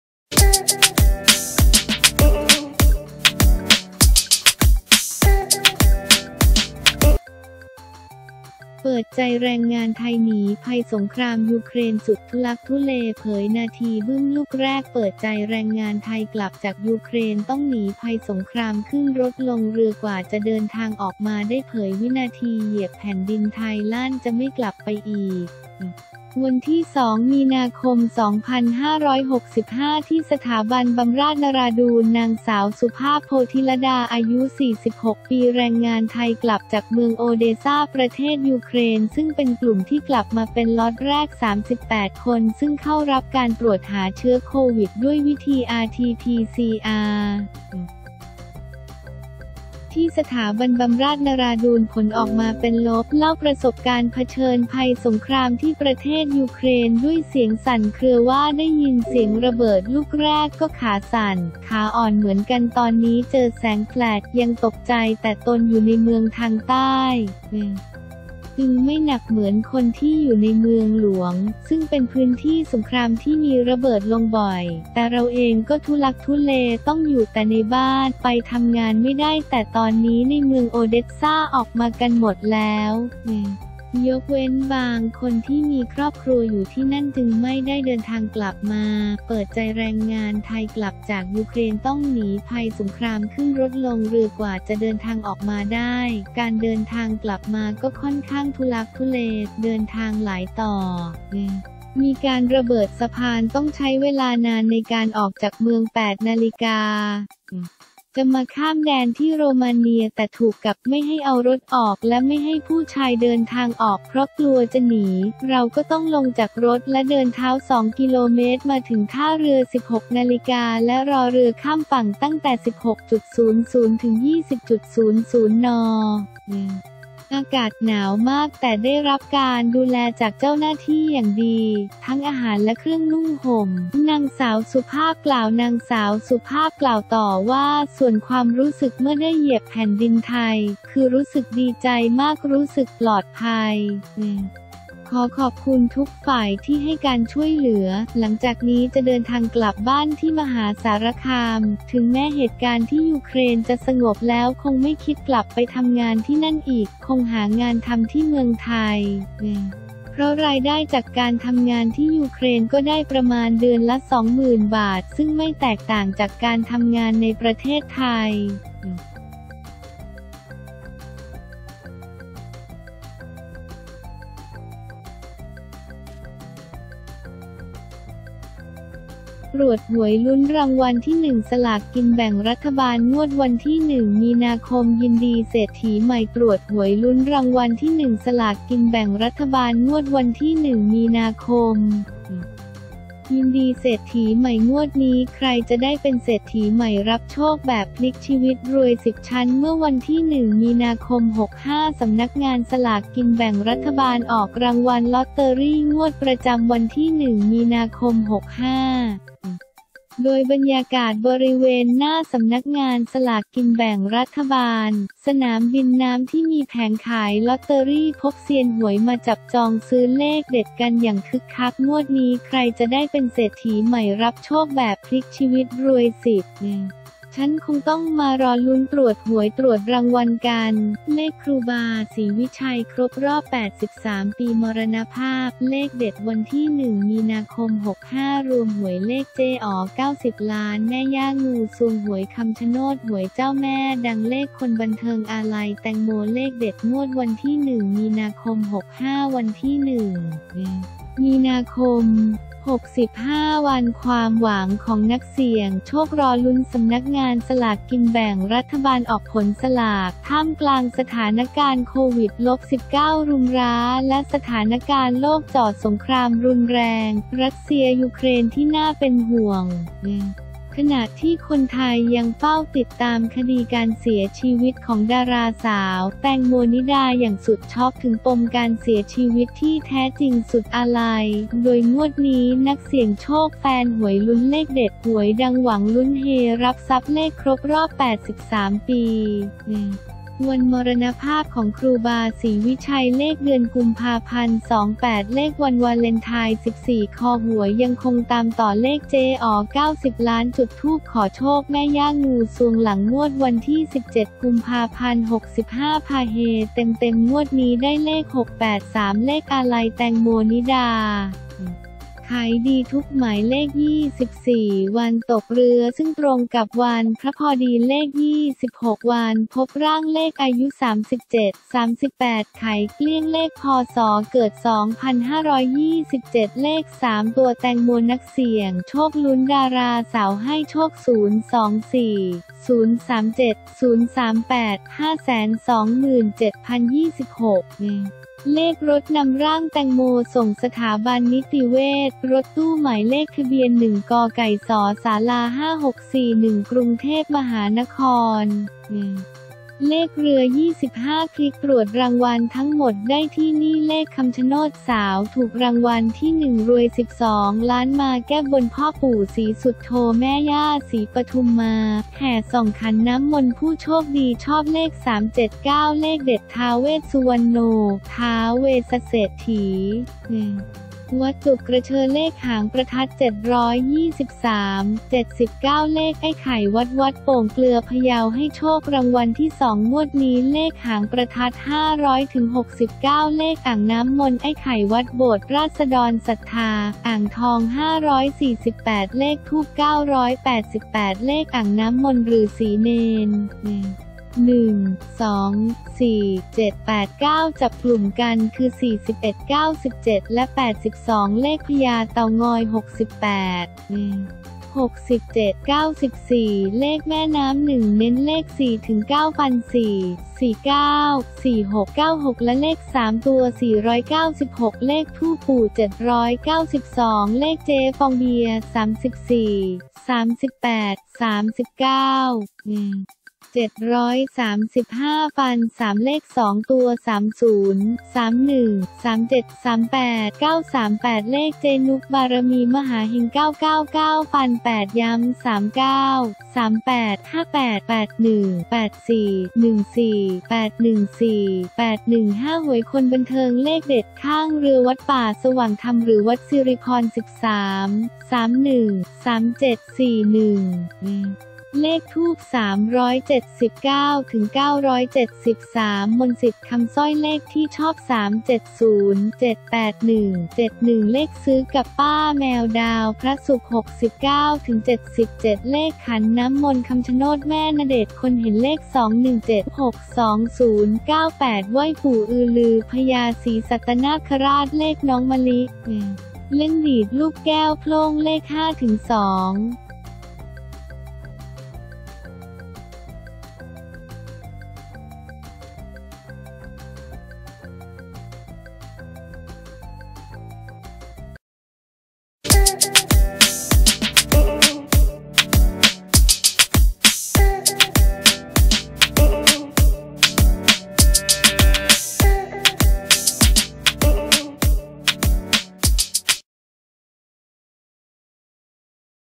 เปิดใจ แรงงานไทย หนีภัยสงครามยูเครน สุดทุลักทุเล เผยนาทีบึ้มลูกแรก
เปิดใจ แรงงานไทย กลับจากยูเครน ต้องหนีภัยสงคราม ขึ้นรถลงเรือ กว่าจะเดินทางออกมาได้ เผยวินาทีเหยียบแผ่นดินไทย ลั่นจะไม่กลับไปอีก
วันที่ 2 มี.ค.2565 ที่สถาบันบำราศนราดูร น.ส.สุภาพ โพธิลเดา อายุ 46 ปี แรงงานไทยกลับจากเมืองโอเดซา ประเทศยูเครน ซึ่งเป็นกลุ่มที่กลับมาเป็นล็อตแรก 38 คน ซึ่งเข้ารับการตรวจหาเชื้อโควิดด้วยวิธี RT-PCR ที่สถาบันบําราศนราดูรผลออกมาเป็นลบ เล่าประสบการณ์เผชิญภัยสงคราม ที่ประเทศยูเครน ด้วยเสียงสั่นเครือ ว่า ได้ยินเสียงระเบิดลูกแรกก็ขาสั่น ขาอ่อนเหมือนกัน ตอนนี้เจอแสงแฟลชยังตกใจ แต่ตนอยู่ในเมืองทางใต้ จึงไม่หนักเหมือนคนที่อยู่ในเมืองหลวง ซึ่งเป็นพื้นที่สงครามที่มีระเบิดลงบ่อย แต่เราเองก็ทุลักทุเลต้องอยู่แต่ในบ้าน ไปทำงานไม่ได้ แต่ตอนนี้ในเมืองโอเดสซาออกมากันหมดแล้ว ยกเว้นบางคนที่มีครอบครัวอยู่ที่นั่นจึงไม่ได้เดินทางกลับมา
เปิดใจ แรงงานไทย กลับจากยูเครน ต้องหนีภัยสงคราม ขึ้นรถลงเรือ กว่าจะเดินทางออกมาได้
“การเดินทางกลับมาก็ค่อนข้างทุลักทุเล เดินทางหลายต่อ มีการระเบิดสะพาน ต้องใช้เวลานานในการออกจากเมือง 08.00 น. จะมาข้ามแดนที่โรมาเนีย แต่ถูกกักไม่ให้เอารถออก และไม่ให้ผู้ชายเดินทางออกเพราะกลัวจะหนี เราก็ต้องลงจากรถและเดินเท้า 2 กิโลเมตรมาถึงท่าเรือ 16.00 น. และรอเรือข้ามฝั่งตั้งแต่ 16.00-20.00 น. อากาศหนาวมาก แต่ได้รับการดูแลจากเจ้าหน้าที่อย่างดี ทั้งอาหารและเครื่องนุ่งห่ม” น.ส.สุภาพ กล่าว